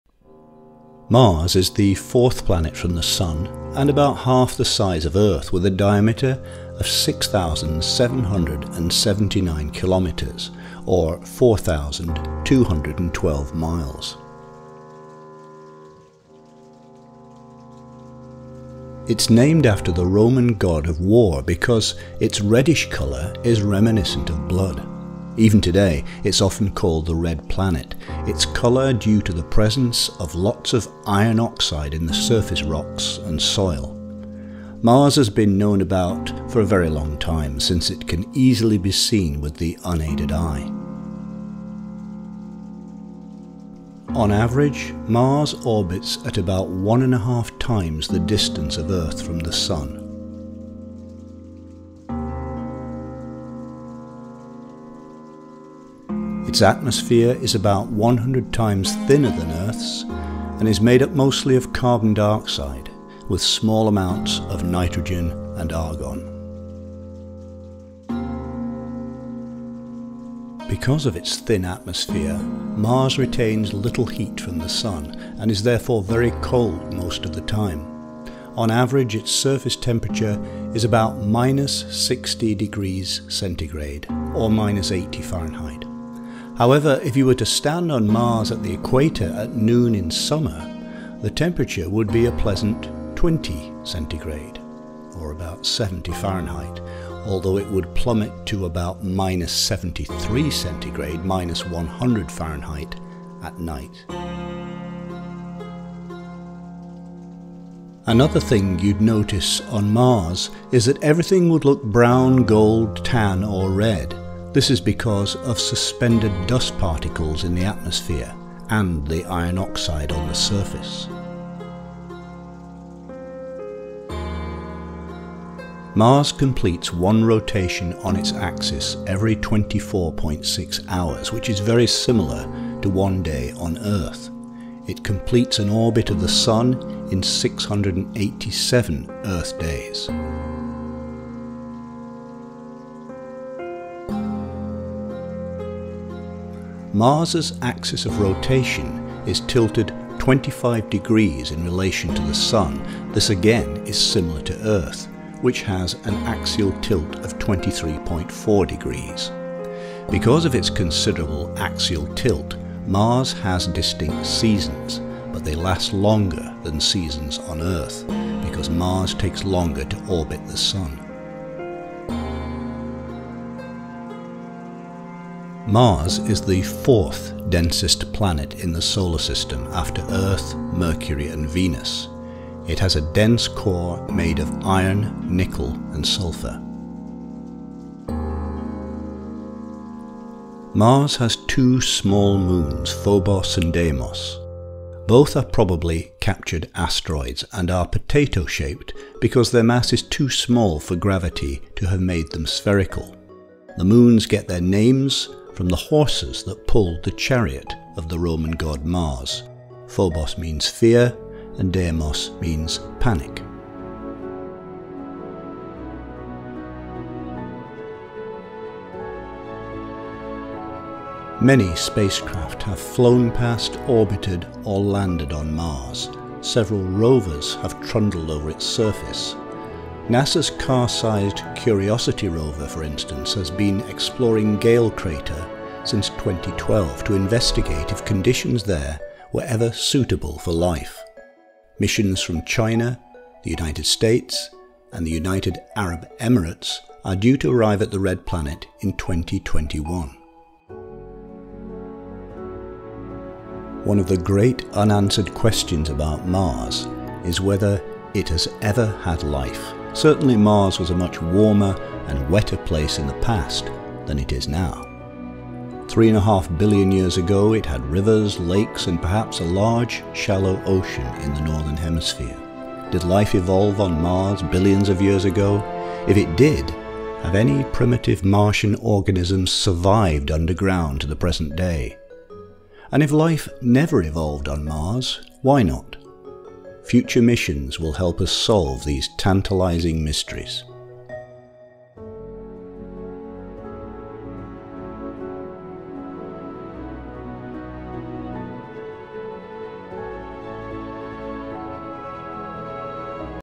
The Planets : Mars
The Red Planet, Mars, fourth from the Sun, has always fascinated us with the question: Did life evolve there?

Images, animations and videos credits:
- NASA
- Wikipedia
- Pixabay